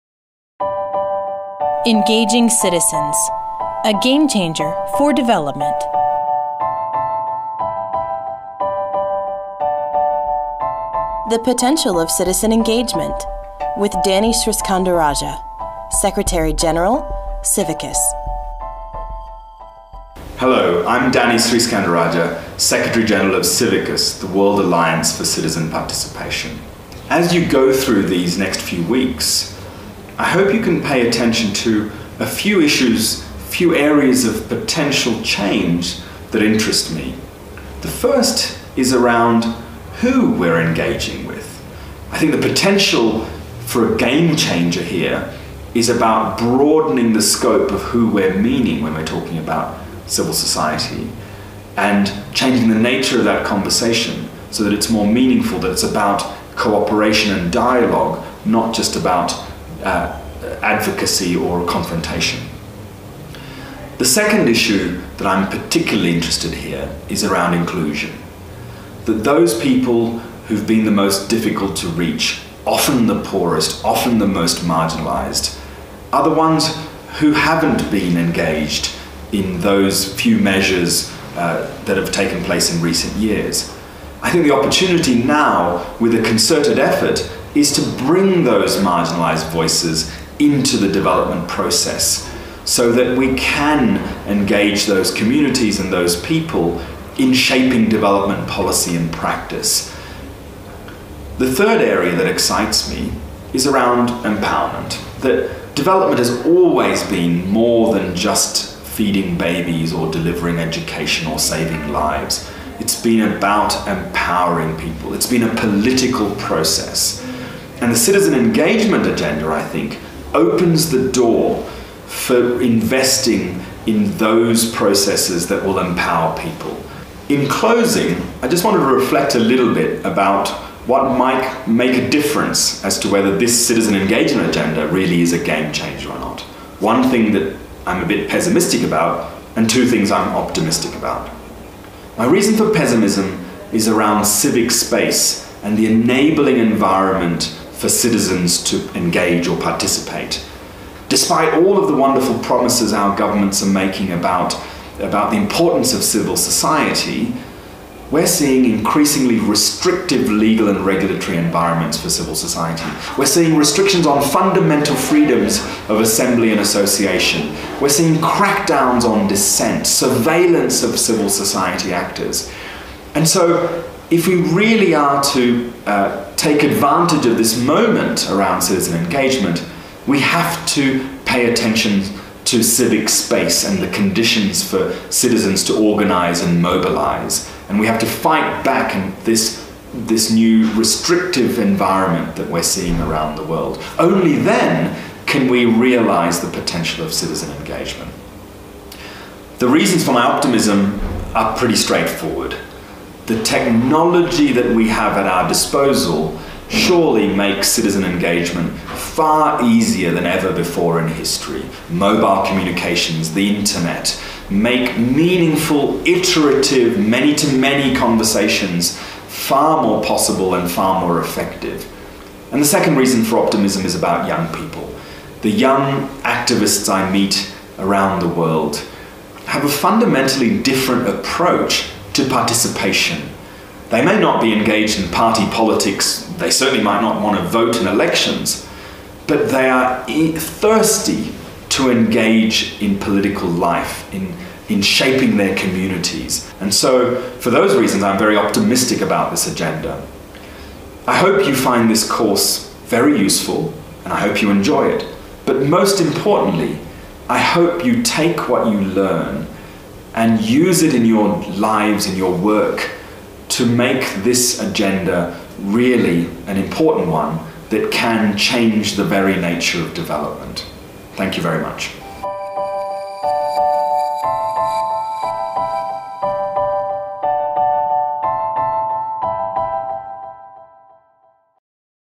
1 3 The Potential of Citizen Engagement 04m 42s
In this short welcome talk, Danny Sriskandarajah delineates three key areas where citizen engagement can have a major impact, and then outlines the main challenges and opportunities in this field.